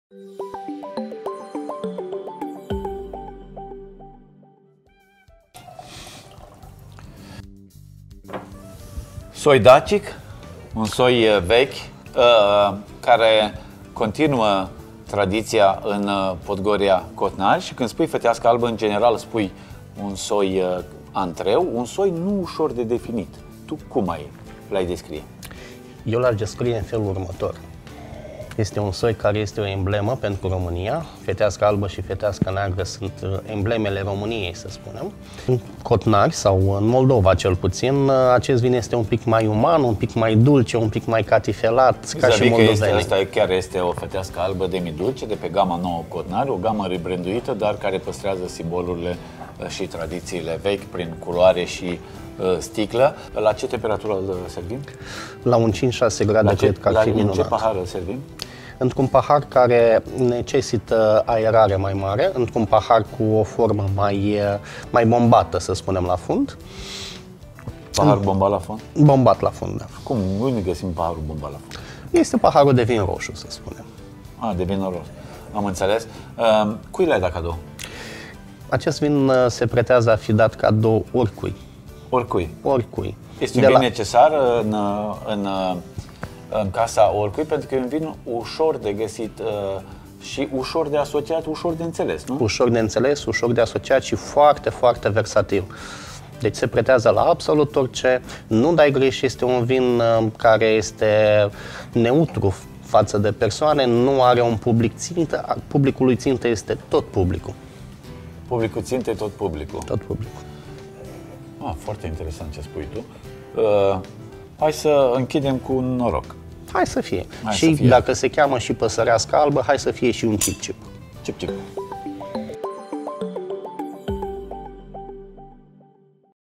Tradițional, Feteasca Albă Demidulce
Feteasca Albă este un soi emblematic al României. În Cotnari, acest soi este mai catifelat exact cum suntem noi, moldovenii.
Hai pe să te bucuri de accentul vinurilor din Cotnari!